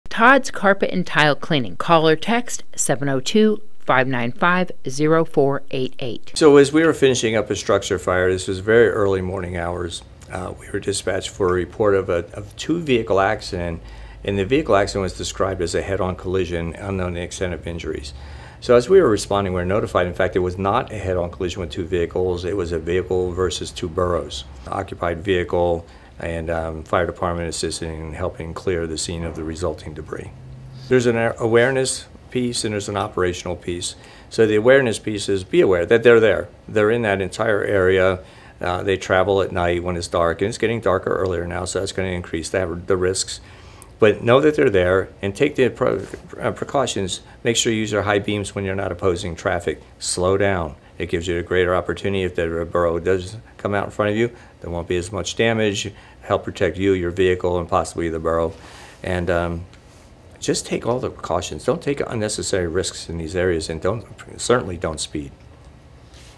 11/01/2022 A Herd of Burros Near the Old Dairy Continue to Cause Havoc for Travelers
Two burros were killed and a dually pick up truck destroyed during a crash recently. Fire Chief Scott Lewis says drivers should drive slowly in certain areas and take some extra precautions to stay safe.